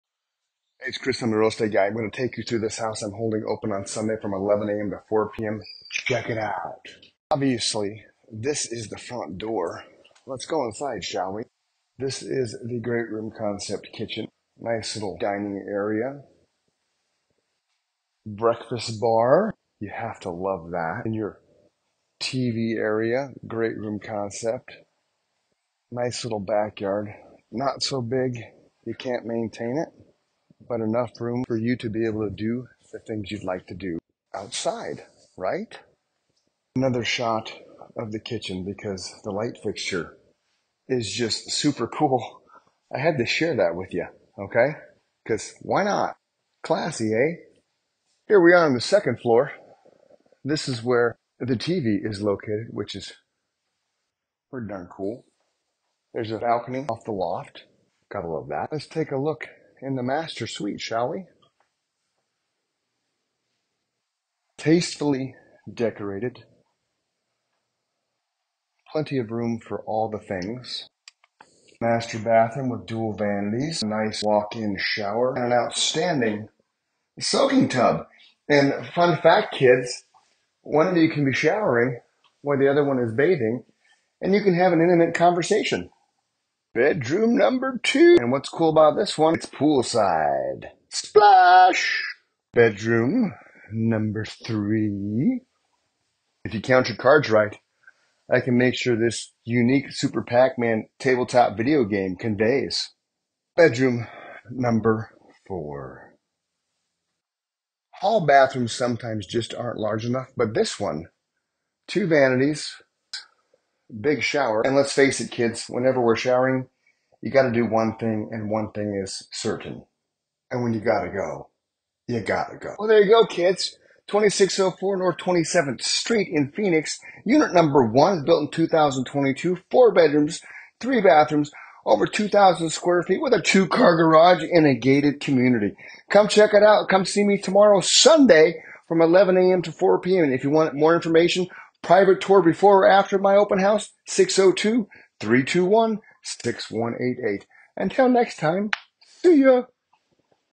2604 N. 27th St., Phoenix, AZ 85008. . rls8guy ￼
Relatively Brand new four-bedroom, three-bath townhouse in Phoenix with modern interior, designer windows, and a two-car garage. Features include a spacious kitchen with an island, dining area, living room, and a backyard with 10 feet of glass. Upstairs has four bedrooms, including a master bedroom with en suite and walk-in closet, plus a loft area.